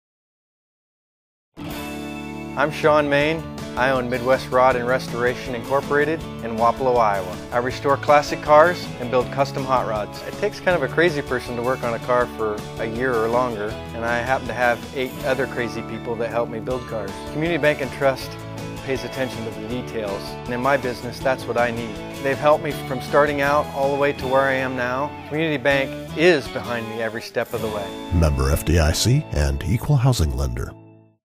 Midwest rod and Restoration / Community Bank Commercial
A commercial running on our local TV stations feturing Midwest Rod and Restoration Inc. A special thanks to our bank "Community Bank" for using us..